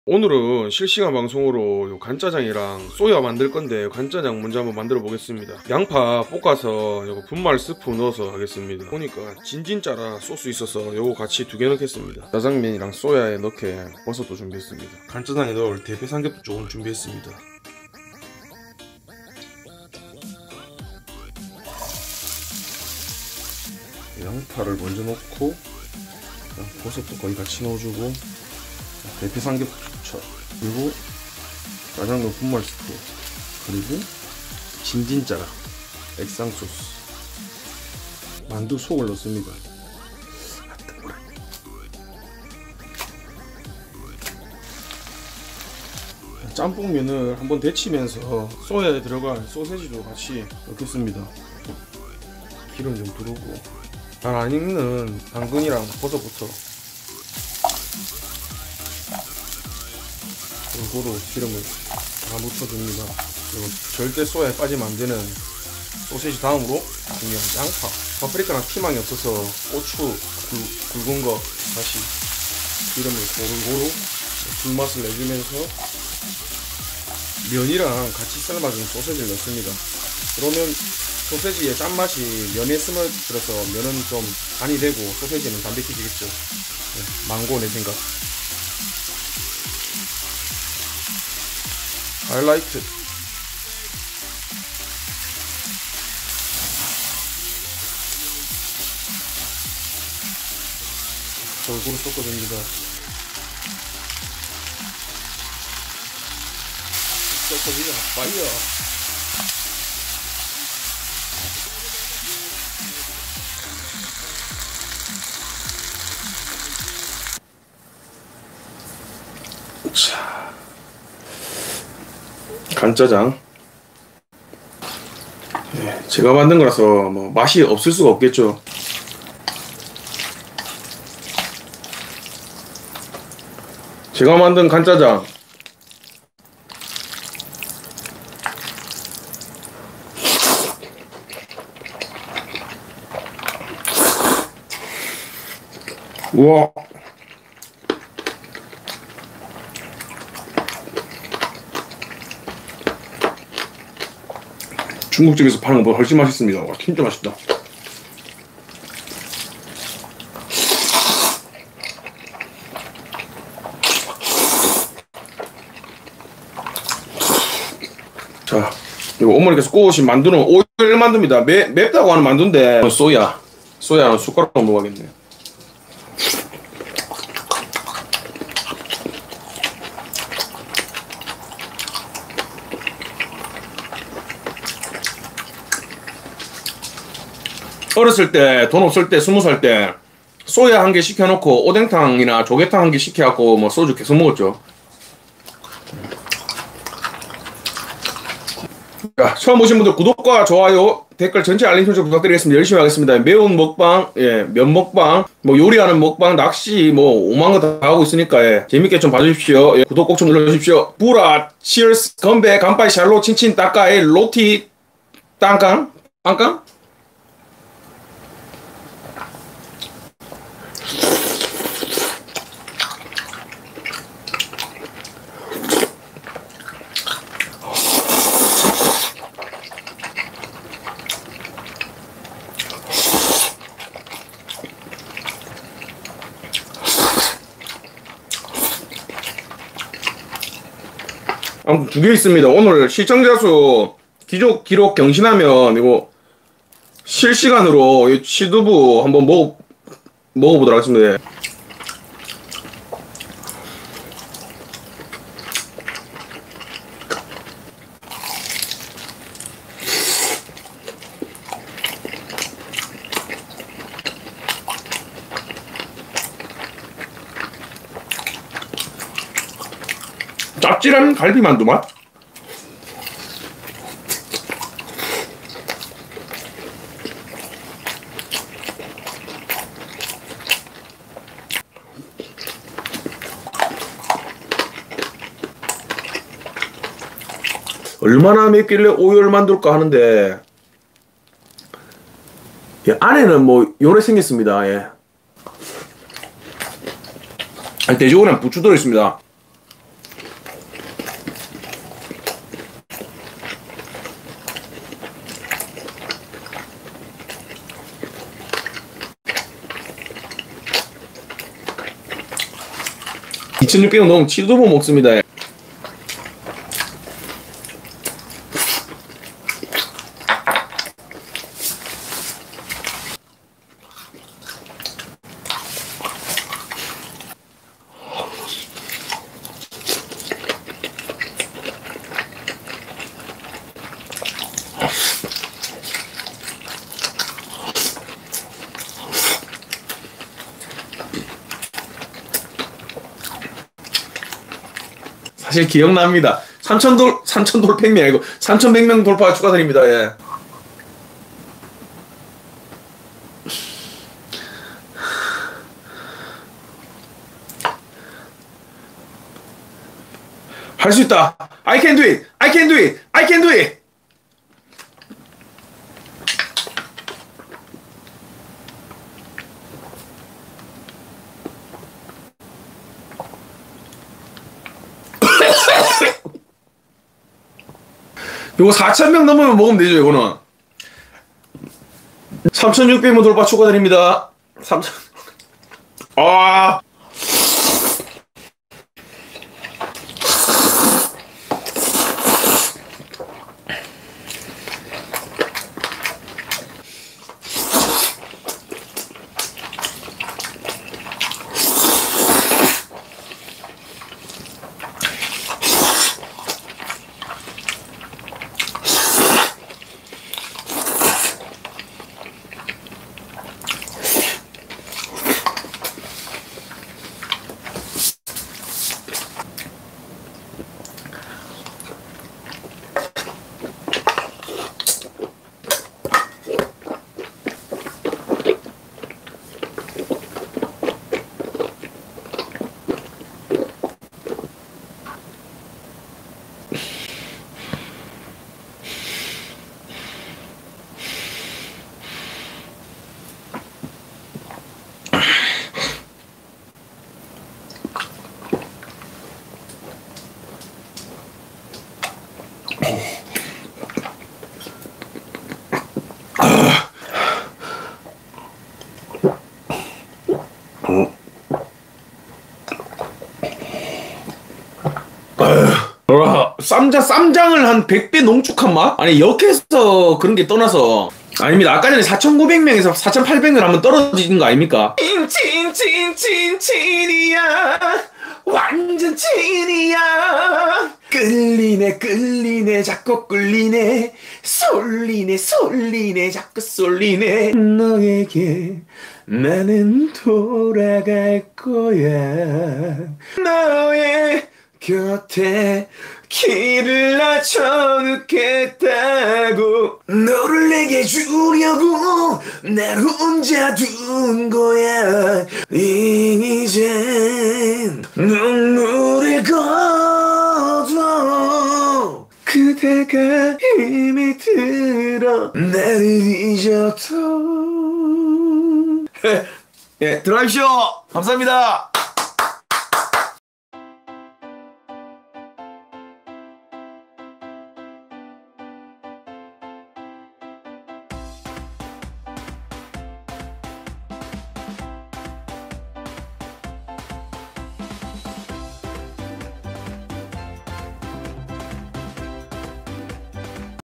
대패삼겹살 간짜장 짬뽕탕 소세지야채볶음 오열만두 취두부 먹방
실시간방송 시청자수 공약으로 인해서... 취두부를 먹었습니다...ㅋㅋ
사놓고 용기가 나지 않아서 못 먹고 있었는데... 실시간방송으로 시청자 4,900분이 보시는데 취두부를 먹게 되었습니다... 진정한 중식 먹방이었네요...ㅎㅎ